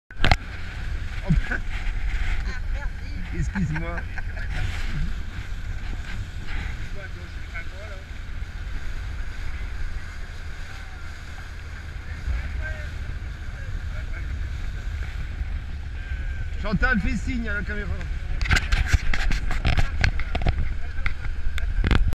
Antartique 2017
Voyage de didier en Antartique et chili 2017